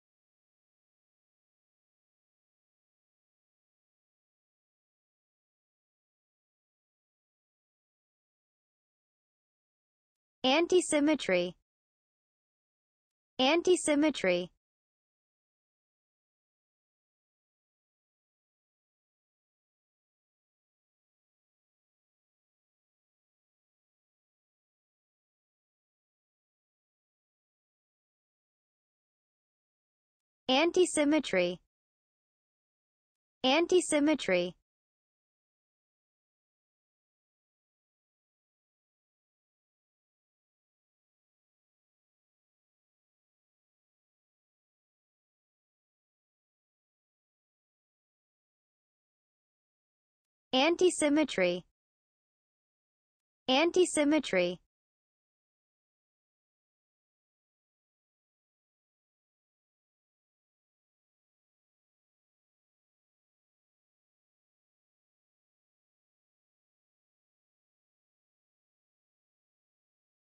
How to say antisymmetry in English? - Pronunciation Owl
How to say antisymmetry in English? Learn the pronunciation of antisymmetry!